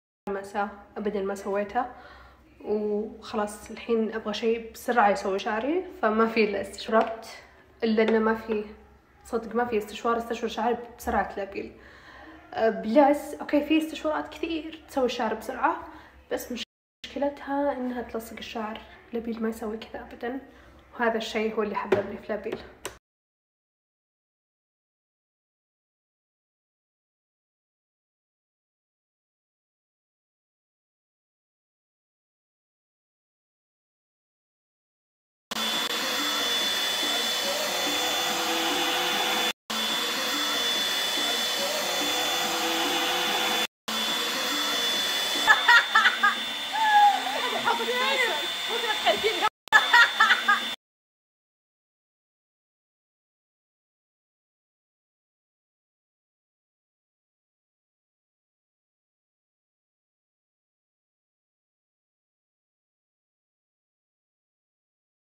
تجربة نجلاء ستايل مع لابيل برش المتميز بسرعته .
لابيل برش غيّرت معايير أجهزة تصفيف الشعر وقدّمت حل مُبتكر يتمثل في جهاز واحد بمزايا مذهلة
يجفف، يصفف، يملّس في عشر دقائق